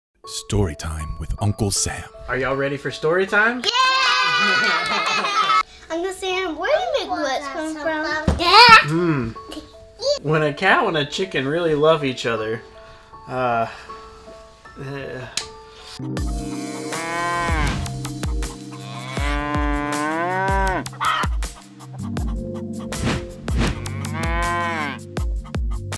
Where does the McWhat? come from?! - Mess Hall - Wilmington, NC
Join us for lunch or dinner in the Cargo District in Downtown Wilmington, NC